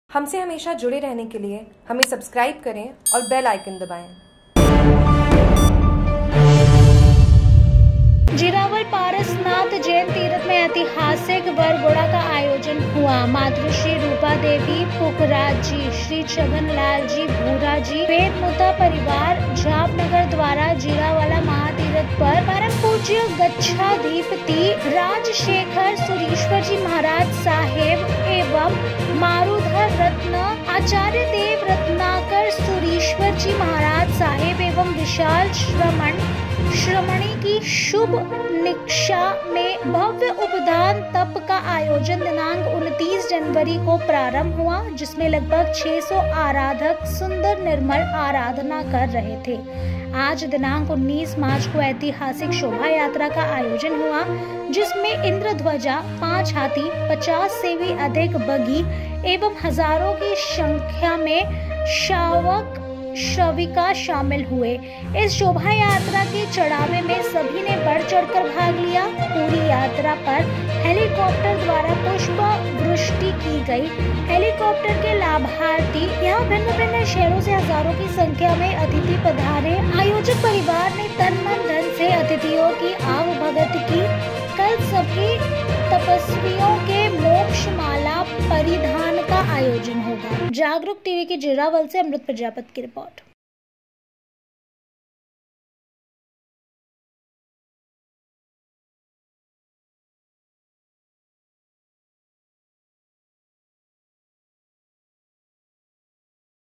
जीरावल : पारसनाथ जैन तीर्थ मैं एतिहासिक वरघोड़ा का आयोजन हुआ
श्री छगनलालजी भुराजी वेदमुथा परिवार झाबनगर ( Marc Computer) द्वारा जीरावला महातीर्थ पर परम पूज्य गच्छाधीपती राजशेखर सूरिश्वरजी महाराज साहेब एवं मरुधर रत्न आचार्यदेव रत्नाकरसूरिश्वर जी महाराज साहेब एवं विशाल श्रमण श्रमणी की शुभ निश्रा में भव्य उपधान तप का आयोजन दिनांक 29 जनवरी को प्रारम्भ हुआ जिसमें लगभग 600 आराधक सुंदर निर्मल आराधना कर रहे थे आज दिनांक 19 मार्च को एतिहासिक शोभायात्रा का आयोजन हुआ जिसमें इंद्रध्वजा,5 हाथी 50 से भी अधिक बगी एवं हज़ारों की शंख्या में श्रावक श्रविका शामिल हुए इस शोभायात्रा के चडावे में सभी ने बढ़ चढ़ कर भाग लिया ।पूरी यात्रा पर हेलीकाप्टर द्वारा पुष्प वृष्टि की गयी हेलीकाप्टर के लाभार्थी श्री गिरधारीलालजी चुन्नीलालजी दोषी थे  यहाँ भिन्न भिन्न शहरों से हज़ारों की संख्या में अतिथि पधारे आयोजक परिवार ने तन मन धन से अतिथियों की आवभगत की कल सभी तपस्वियों के मोक्ष माला परिधान जा आयोजन होगा